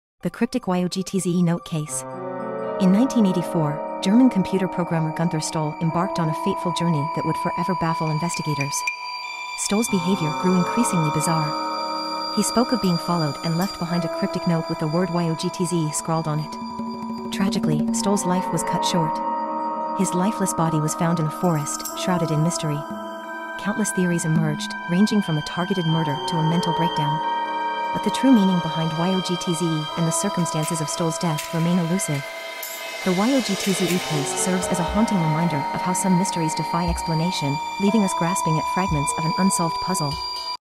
The YOGTZE Case: Cryptic Unsolved | True Crime
Join us as we unravel the perplexing enigma of the YOGTZE Case, a chilling true crime mystery that has baffled investigators for decades. In 1984, German computer programmer Günther Stoll embarked on a fateful journey, leaving behind a trail of strange behavior and a cryptic note. What happened to Stoll and the meaning behind the enigmatic word "YOGTZE" remain unsolved to this day. Prepare to be captivated by the unsolved puzzle that continues to confound and intrigue. Watch now for an immersive journey into the YOGTZE Case.